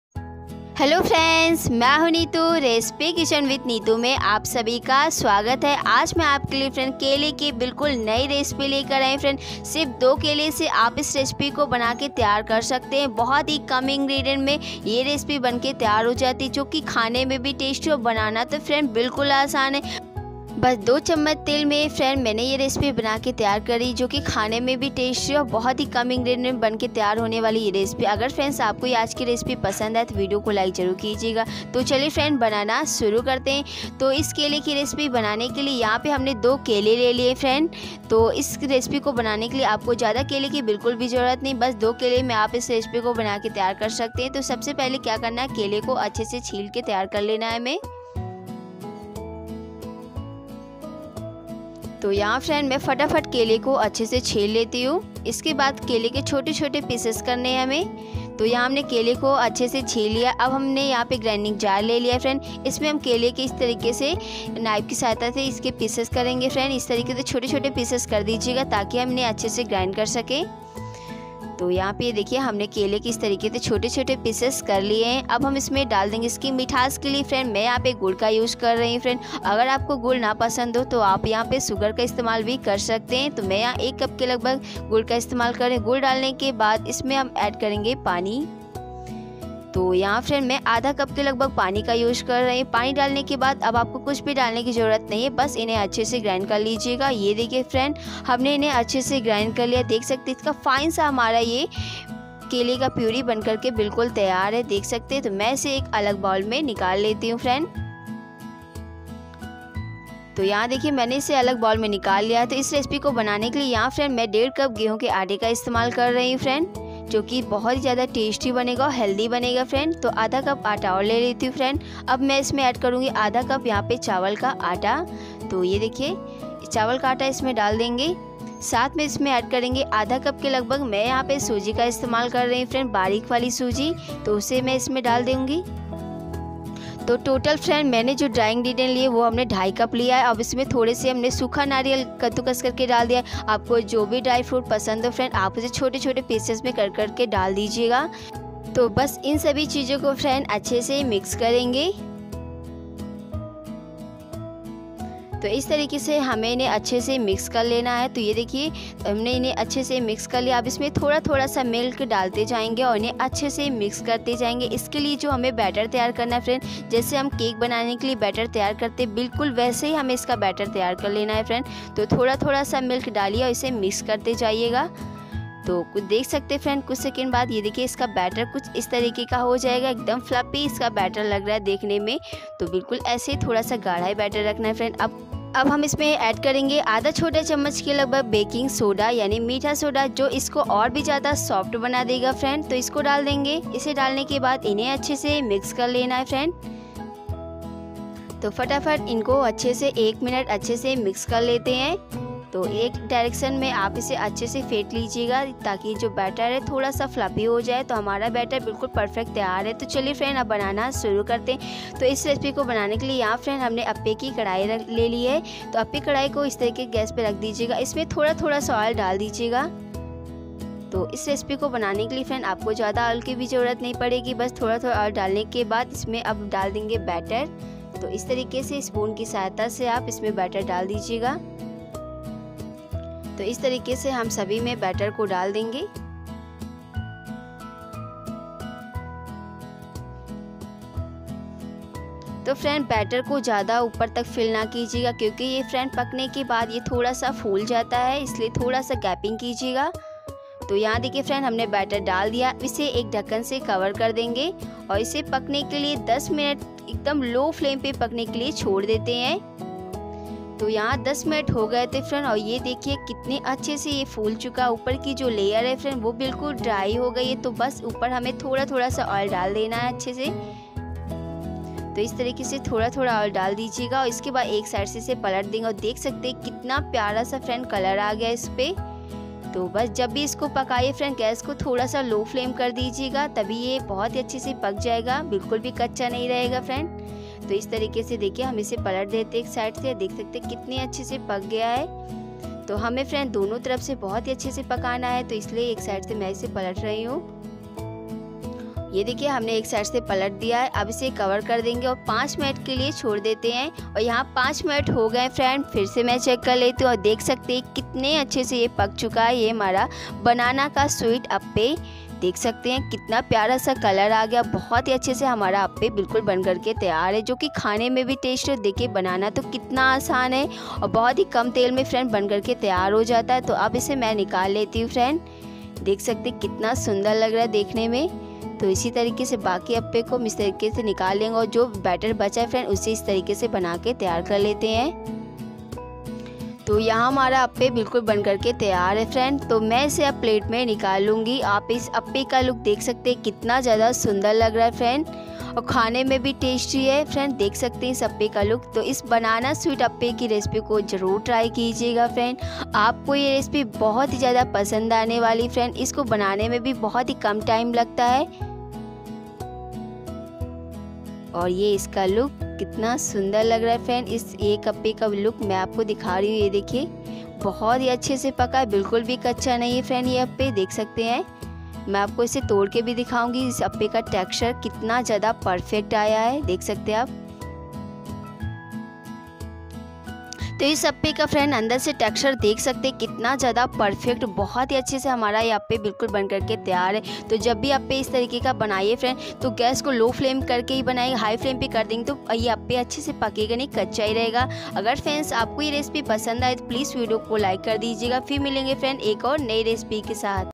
सिर्फ दो केले से बनाए बहुत ही टेस्टी बिना तले हेल्थी स्वीट रेसिपी।Banana sweet appam recipe
Ingredients-
Banana 🍌 - 2
coconut
jaggery
oil
milk
baking soda